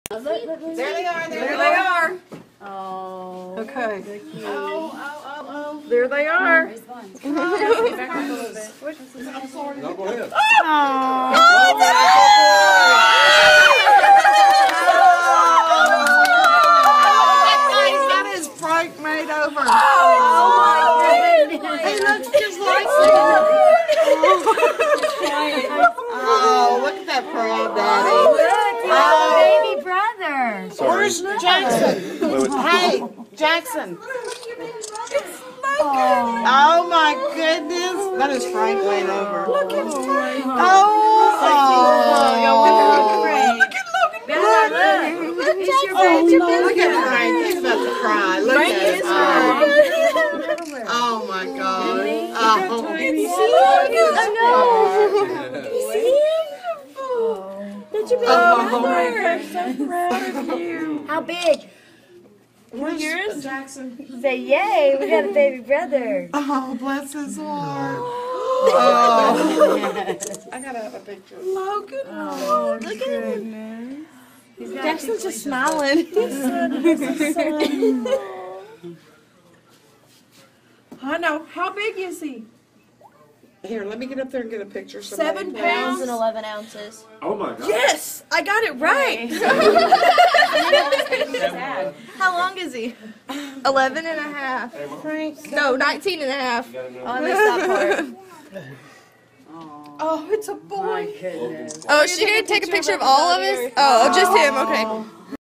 Meet Logan Clark! Gender Reveal!
My future husband announcing the sex of our baby to a waiting room full of family & friends! If you're pregnant I highly suggest NOT finding out the gender of your baby. It was so much fun!